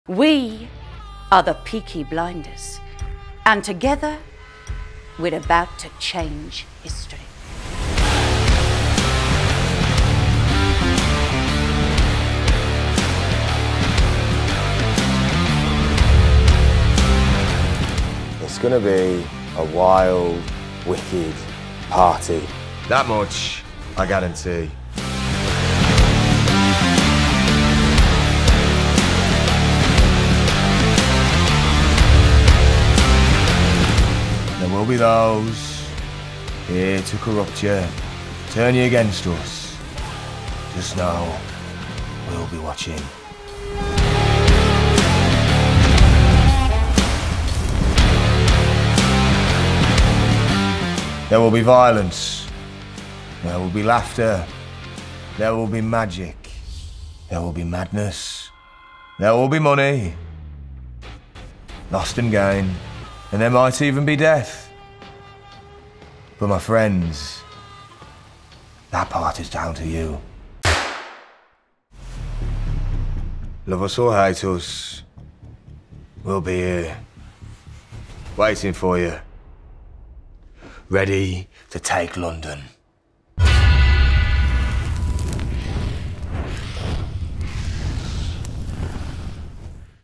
Peaky Blinders: The Rise - Extended Trailer
PEAKY BLINDERS: THE RISE is "a truly unmissable experience" (Express) that transports you inside the world of TV’s most notorious drama at the Camden Garrison, the glamorously gritty new venue in the heart of Camden Market.

Will Tommy Shelby and his family succeed in their ambition to take over London? It’s all up to you, as you explore the live sets, complete the challenges and interact with the show’s most iconic characters. Whether you’re a devoted fan or just up for a brilliant night out, dust off your finest flat cap and get ready for "a boisterous good time!" (WhatsOnStage).